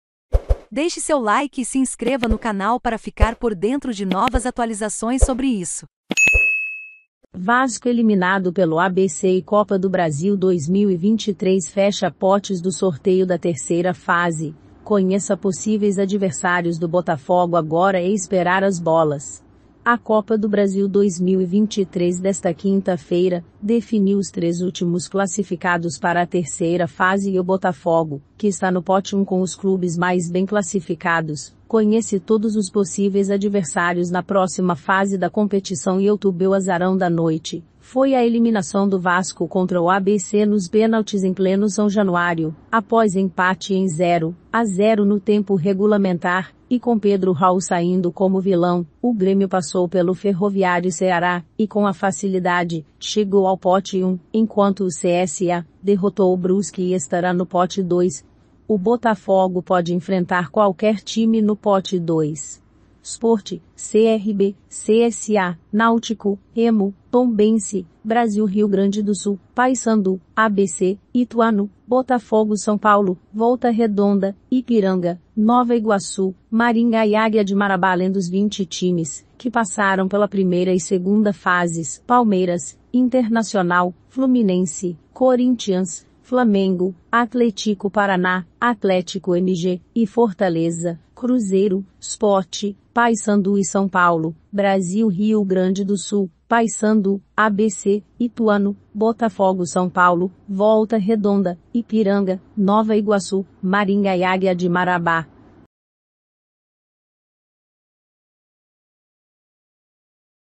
VASCO ELIMINADO PELO ABC E COPA DO BRASIL 2023 FECHA POTES DO SORTEIO DA 3ª FASE;...
Vasco eliminado pelo ABC e Copa do Brasil 2023 fecha potes do sorteio da 3ª fase; conheça possíveis adversários do Botafogo

YouTubeO azarão da noite foi a derrota do Vasco para o ABC nos pênaltis em pleno São Januário. após empate em 0 a 0 no tempo regulamentar e com Pedro Raul saindo como o vilão.